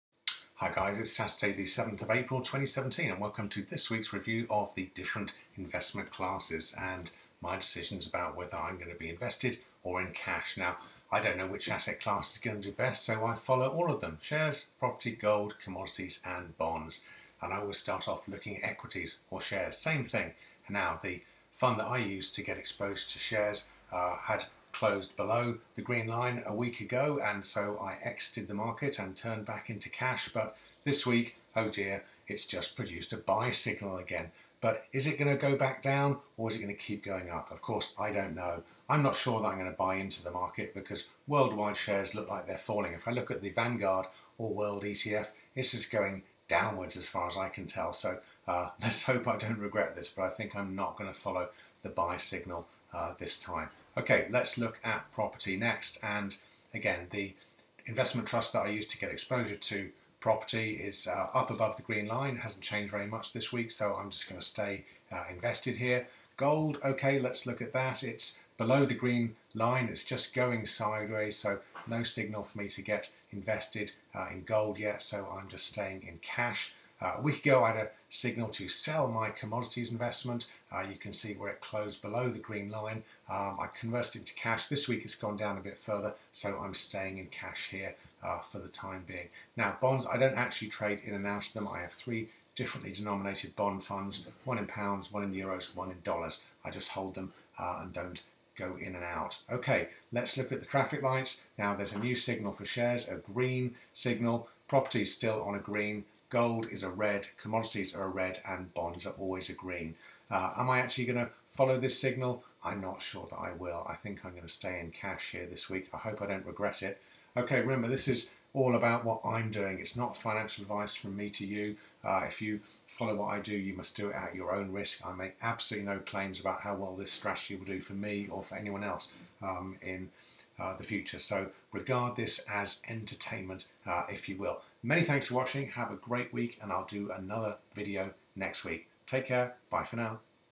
A new BUY signal!
Irritatingly, I have a new signal to BUY equities, just  a few days after having liquidated my position. Will I take the signal?...